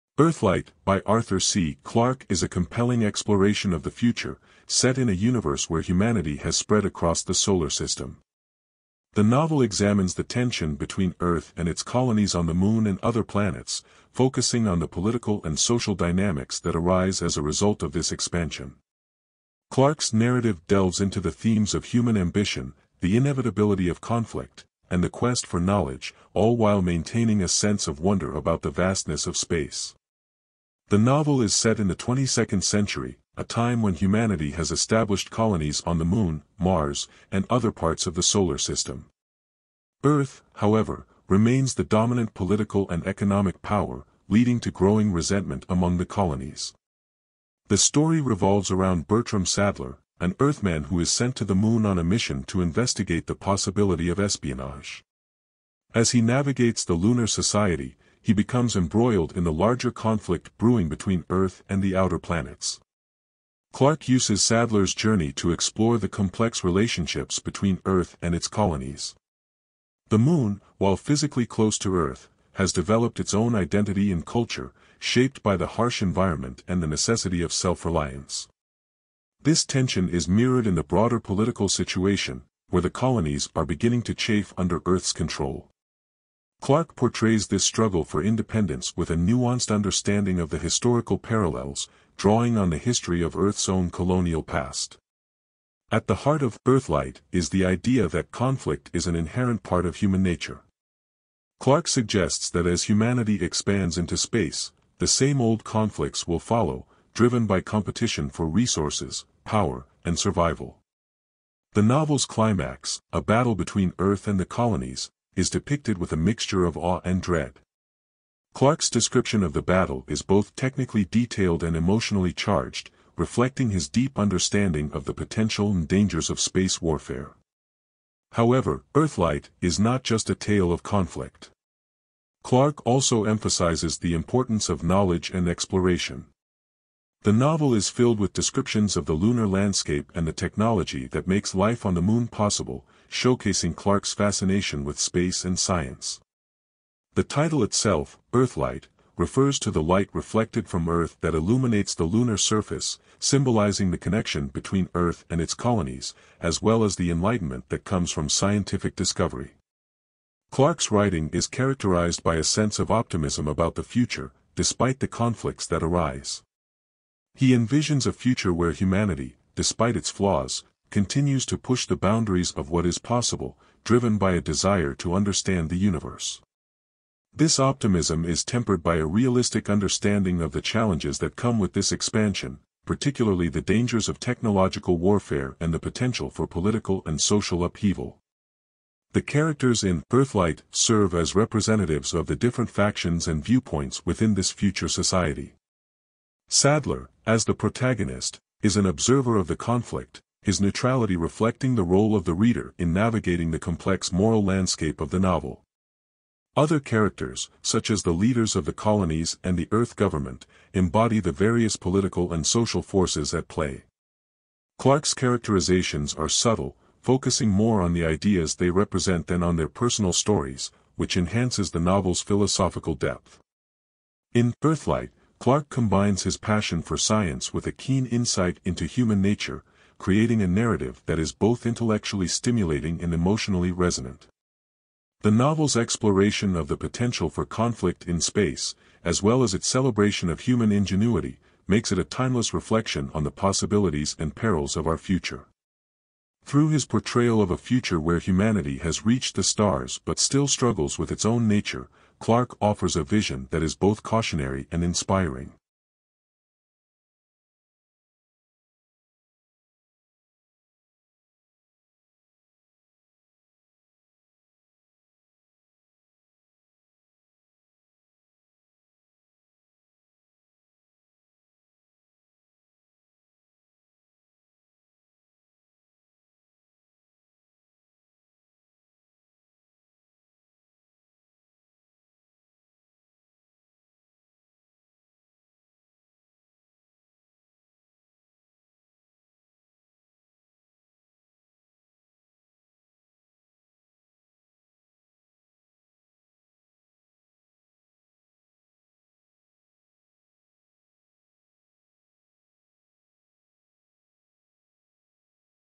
"Earthlight" By Arthur C. Clarke
"Earthlight" by Arthur C. Clarke is a compelling exploration of the future, set in a universe where humanity has spread across the solar system. The novel examines the tension between Earth and its colonies on the Moon and other planets, focusing on the political and social dynamics that arise as a result of this expansion. Clarke’s narrative delves into the themes of human ambition, the inevitability of conflict, and the quest for knowledge, all while maintaining a sense of wonder about the vastness of space.The novel is set in the 22nd century, a time when humanity has established colonies on the Moon, Mars, and other parts of the solar system. Earth, however, remains the dominant political and economic power, leading to growing resentment among the colonies. The story revolves around Bertram Sadler, an Earthman who is sent to the Moon on a mission to investigate the possibility of espionage. As he navigates the lunar society, he becomes embroiled in the larger conflict brewing between Earth and the outer planets.Clarke uses Sadler’s journey to explore the complex relationships between Earth and its colonies. The Moon, while physically close to Earth, has developed its own identity and culture, shaped by the harsh environment and the necessity of self-reliance. This tension is mirrored in the broader political situation, where the colonies are beginning to chafe under Earth’s control. Clarke portrays this struggle for independence with a nuanced understanding of the historical parallels, drawing on the history of Earth’s own colonial past.At the heart of "Earthlight" is the idea that conflict is an inherent part of human nature. Clarke suggests that as humanity expands into space, the same old conflicts will follow, driven by competition for resources, power, and survival. The novel’s climax, a battle between Earth and the colonies, is depicted with a mixture of awe and dread. Clarke’s description of the battle is both technically detailed and emotionally charged, reflecting his deep understanding of the potential and dangers of space warfare.However, "Earthlight" is not just a tale of conflict. Clarke also emphasizes the importance of knowledge and exploration. The novel is filled with descriptions of the lunar landscape and the technology that makes life on the Moon possible, showcasing Clarke’s fascination with space and science. The title itself, "Earthlight," refers to the light reflected from Earth that illuminates the lunar surface, symbolizing the connection between Earth and its colonies, as well as the enlightenment that comes from scientific discovery.Clarke’s writing is characterized by a sense of optimism about the future, despite the conflicts that arise. He envisions a future where humanity, despite its flaws, continues to push the boundaries of what is possible, driven by a desire to understand the universe. This optimism is tempered by a realistic understanding of the challenges that come with this expansion, particularly the dangers of technological warfare and the potential for political and social upheaval.The characters in "Earthlight" serve as representatives of the different factions and viewpoints within this future society. Sadler, as the protagonist, is an observer of the conflict, his neutrality reflecting the role of the reader in navigating the complex moral landscape of the novel. Other characters, such as the leaders of the colonies and the Earth government, embody the various political and social forces at play. Clarke’s characterizations are subtle, focusing more on the ideas they represent than on their personal stories, which enhances the novel’s philosophical depth.In "Earthlight," Clarke combines his passion for science with a keen insight into human nature, creating a narrative that is both intellectually stimulating and emotionally resonant. The novel’s exploration of the potential for conflict in space, as well as its celebration of human ingenuity, makes it a timeless reflection on the possibilities and perils of our future. Through his portrayal of a future where humanity has reached the stars but still struggles with its own nature, Clarke offers a vision that is both cautionary and inspiring.